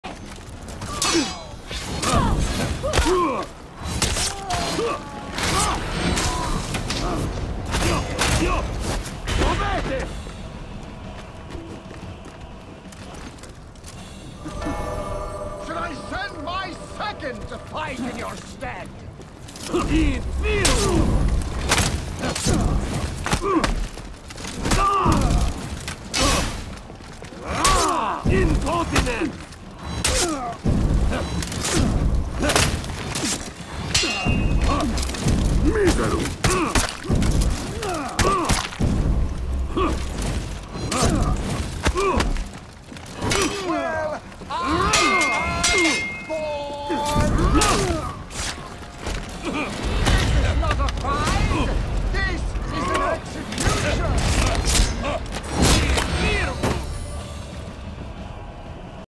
Watch the fake peacekeeper
This guy kept on trying to sidestep my punches and kept getting slapped with my GB fakes